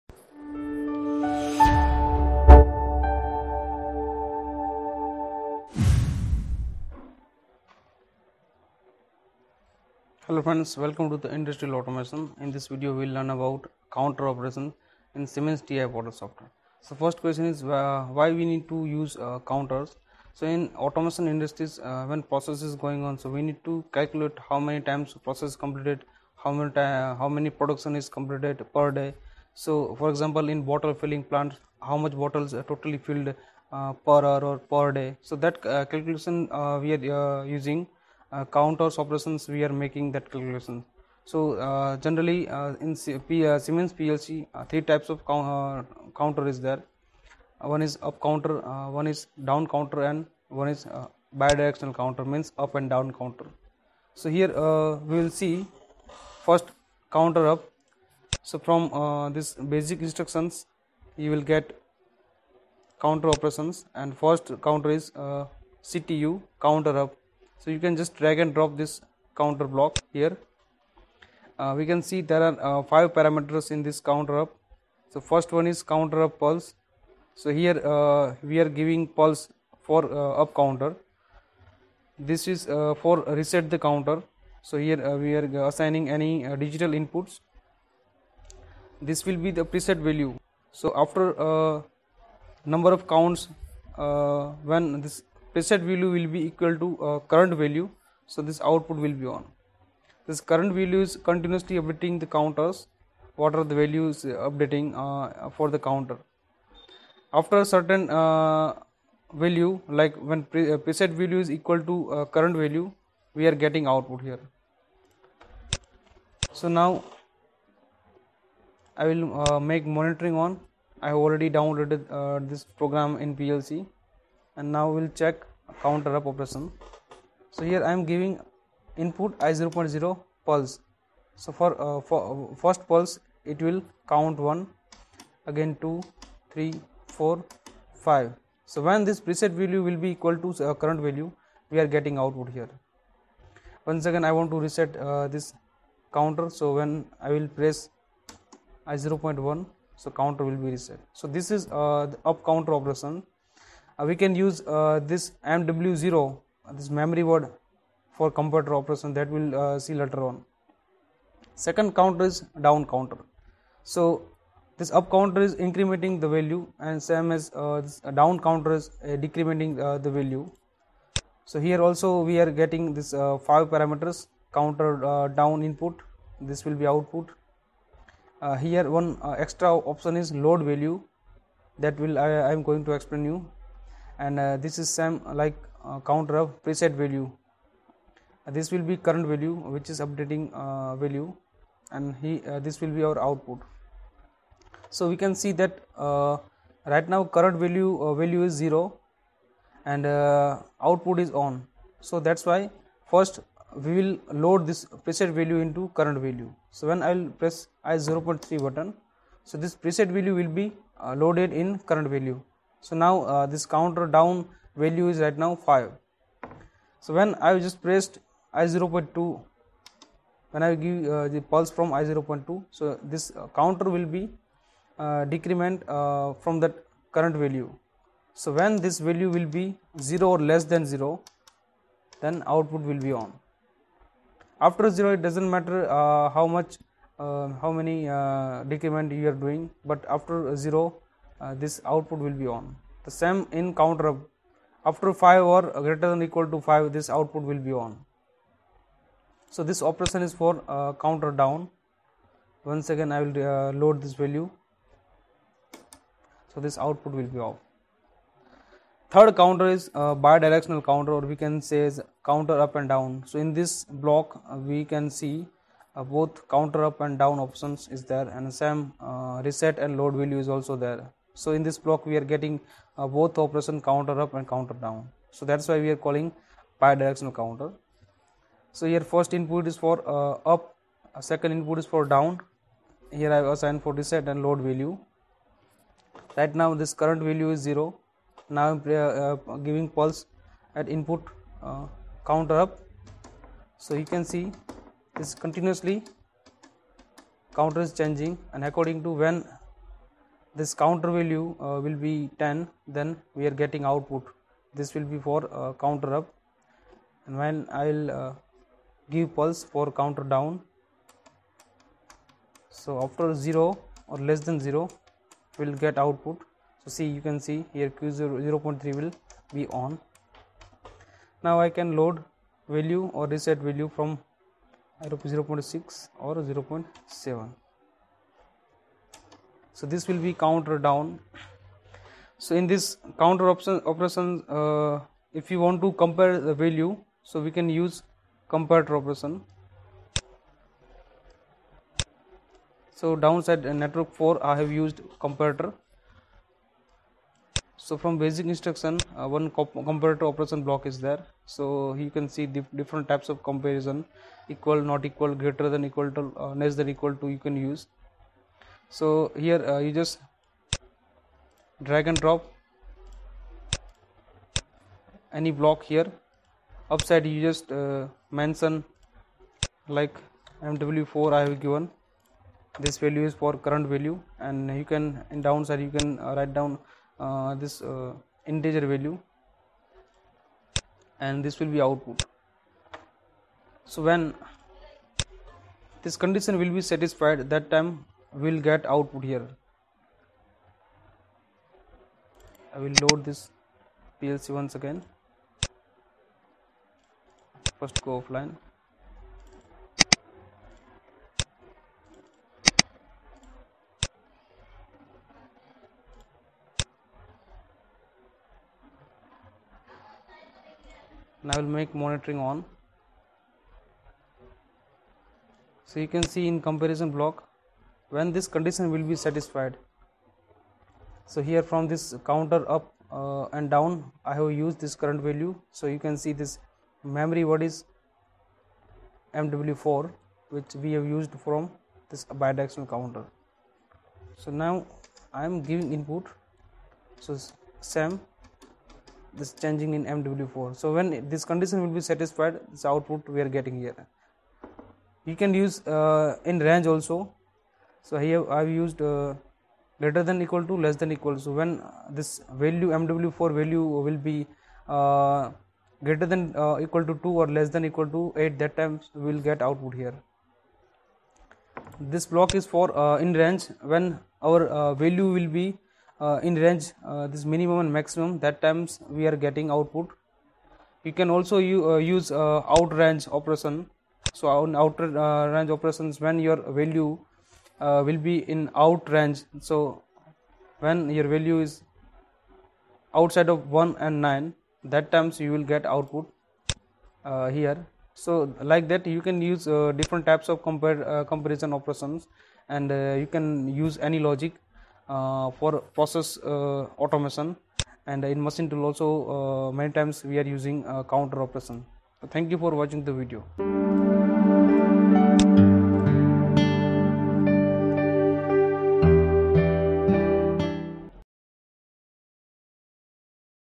PLC Tutorial 5 : Counter Operations in PLC
SIEMENS S7 PLC series Counters Operations with comparative instructions Explanation with examplels in TIA portal

For any more details please visit our below links.